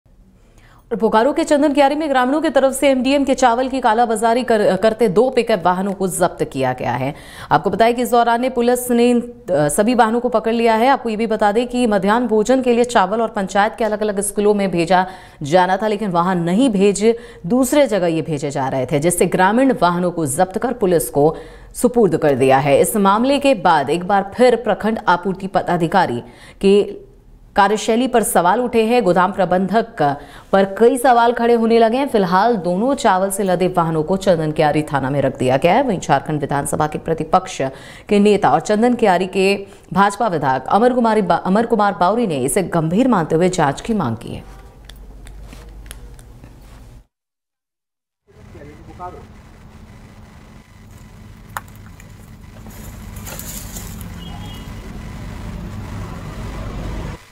बोकारो: मध्यान्ह भोजन मे चावल का कालाबजारी, ग्रामीणों ने दो वाहन किया पुलिस के हवाले
live7BHARAT
jmm news
Ranchi update
Jharkhand update
Congress News
BJP news
Cm viral news
cm news
breaking news